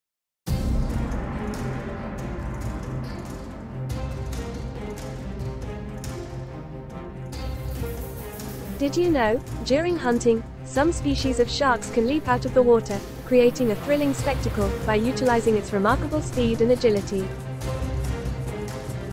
Silent Predators: Unleashing the Ocean's Ultimate Hunting Machines
Discover the awe-inspiring world of the ocean's most formidable hunters in "Silent Predators: Unleashing the Ocean's Ultimate Hunting Machines." Brace yourself for an immersive journey as we delve deep into the realm of these apex predators, exploring their stealth, power, and unmatched hunting skills. Witness the breath-taking precision and agility of these underwater marvels as they navigate the vast expanses of the ocean, utilizing their razor-sharp instincts and lightning-fast movements to stalk and capture their prey.